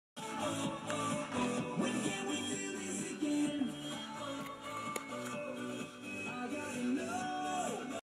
KALE PECAN CRANBERRY SALAD MIX marketside bag
Trying the Salad in the bag from marketplace. summer time salads.